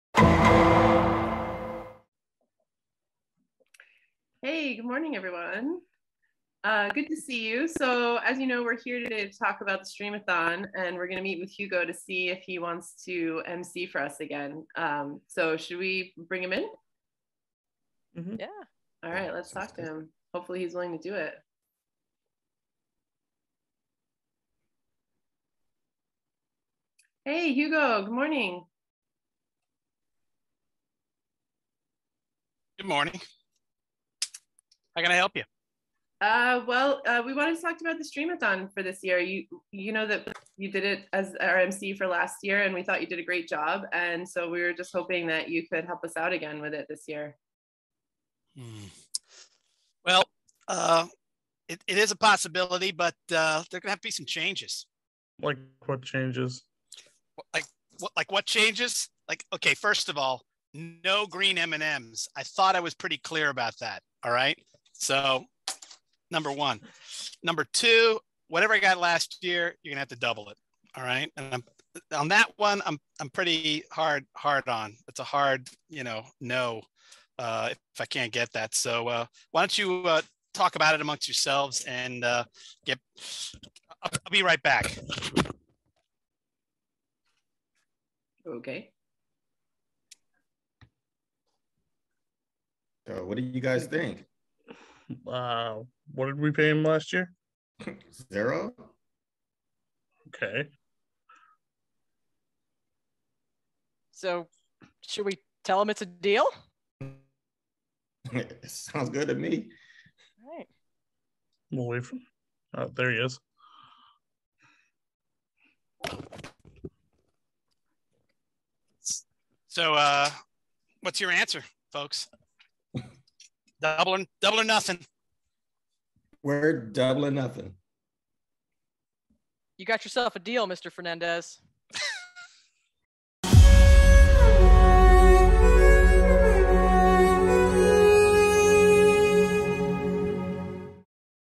LaGCC Faculty Council Stream-a-thon 2022 Promo: Meeting with Talent
The Committee meets to convince Hugo to host our event once more.

The Stream-a-thon is coming...
April 28th, 2022
Facebook.com/LaGFacultyCouncil